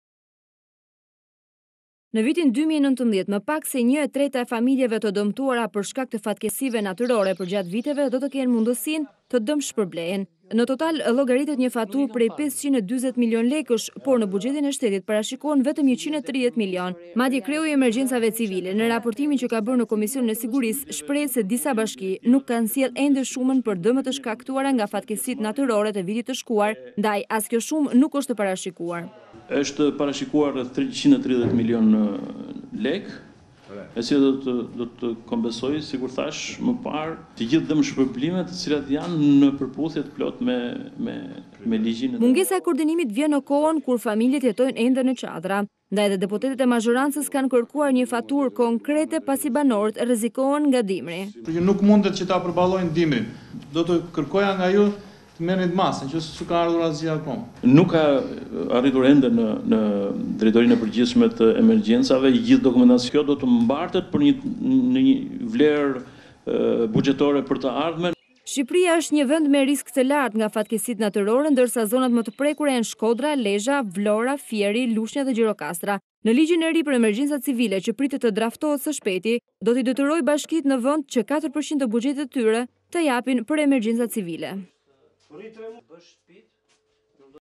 Ora News - Dëmshpërblimet nga fatkeqësitë natyrore, Çako: Janë parashikuar 130 mln lekë
Në vitin 2019 më pak se një e treta e familjeve të dëmtuara për shkak të fatkeqësive natyrore përgjatë viteve do të ketë mundësinë që të dëmshpërblehet.

Në total llogaritet një faturë prej 540 milionë lekësh, por në buxhetin e shtetit parashikohen vetëm 130 milionë. Madje Kreu i Emergjencave Civile Haki Çako në raportimin që ka bërë në Komisionin e Sigurisë shprehet se  disa Bashki nuk kanë sjellë shumën për dëmet e shkaktuara nga fatkeqësitë natyrore të vitit të shkuar, ndaj as kjo shumë nuk është e parashikuar.

Çako: Është parashikuar rreth 130 mln lekë, e cila do të kompesojë të gjithë dëmshpërblimet që janë në përputhje të plotë me ligjin.

Mungesa e koordinimit vjen në kohën familjet jetojnë ende në çadra. Ndaj dhe deputetët e mazhorancës kanë kërkuar një faturë konkrete pasi banorët rrezikohen nga dimri.